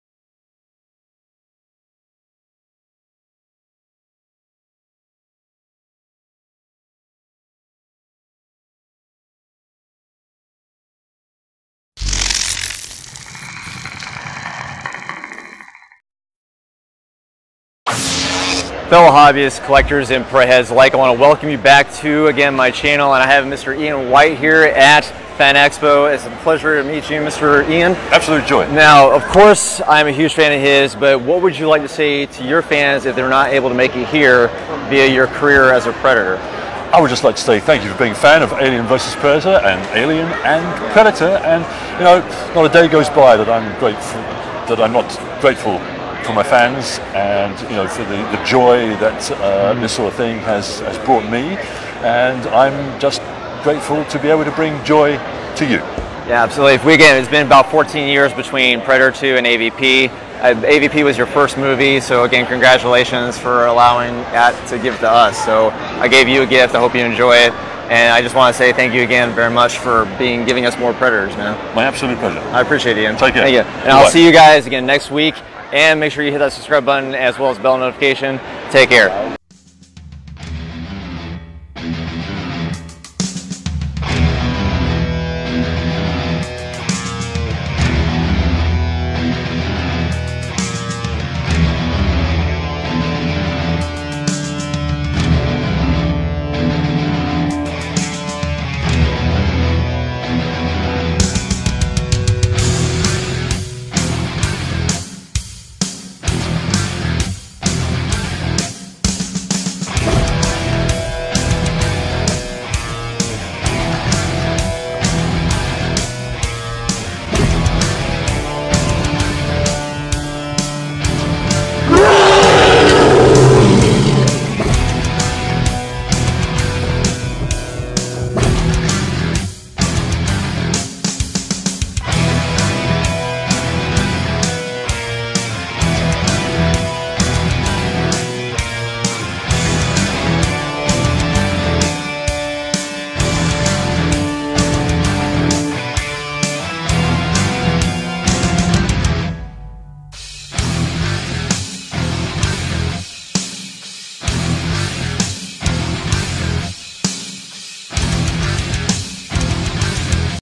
A Predator "Thank You" from Ian Whyte
I finally get to meet the Predator actor Mr. Ian Whyte who plays both Scar / Wolf as main Predators in both Alien vs Predator and Alien  vs. Predator: Requiem.